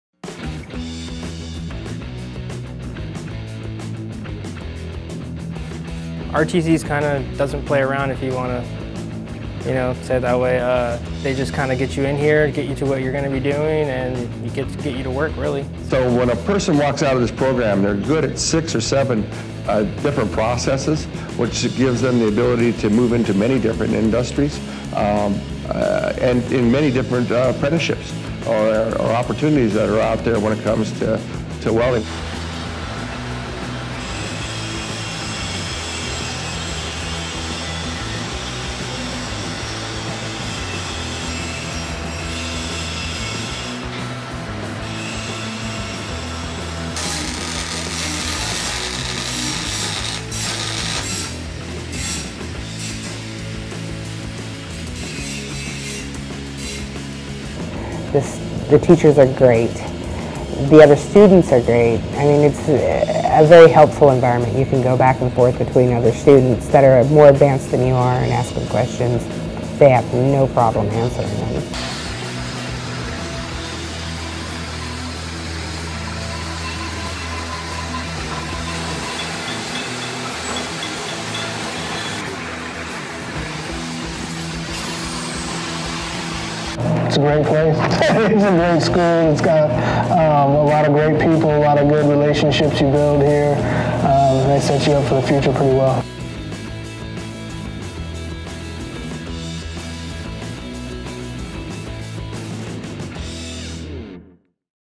Welding at Renton Technical College
This program prepares welders for most phases of the industry. It combines classroom time with practical experience in the most common manual and semi-automatic welding processes: OFW (gas), SMAW, GMAW, FCAW, GTAW, SAW (arc), MIG, and TIG. Graduates can earn certifications from the state and from the Washington Association of Building Officials.